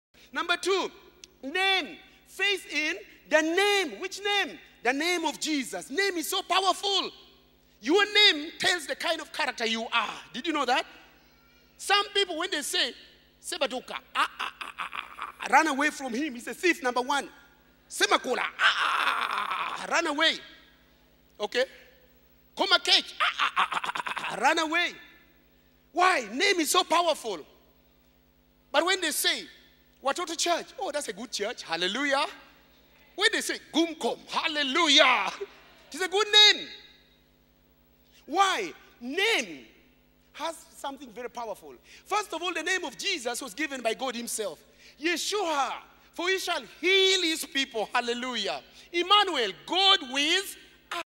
Power Released
Ps. Chris talks about an amazing reality of the power and authority that every believer has been given which we should exercise... to listen to the entire sermon, log